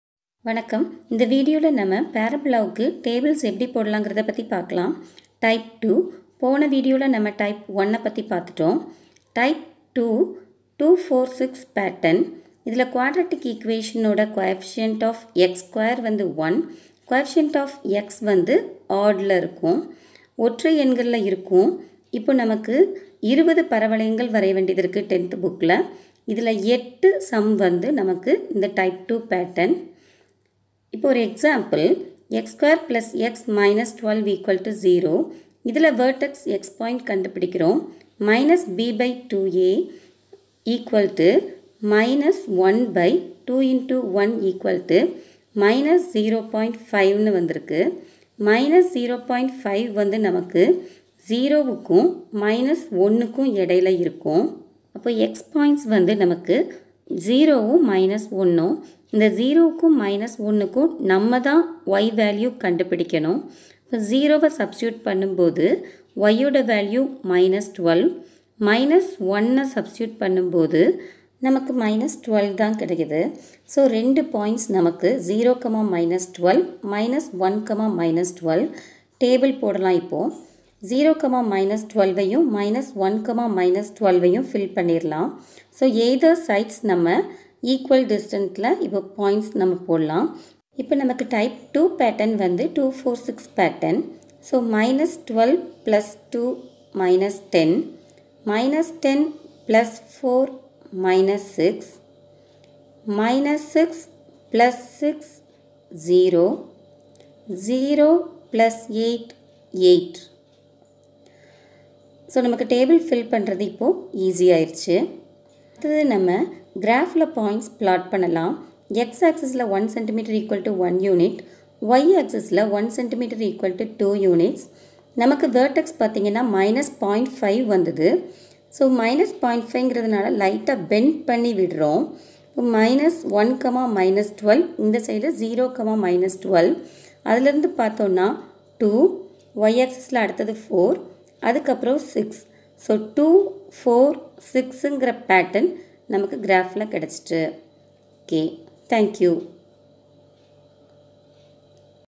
10th std graph - Type 2(2-4-6 pattern)
Tables for parabola - Easy method for slow learners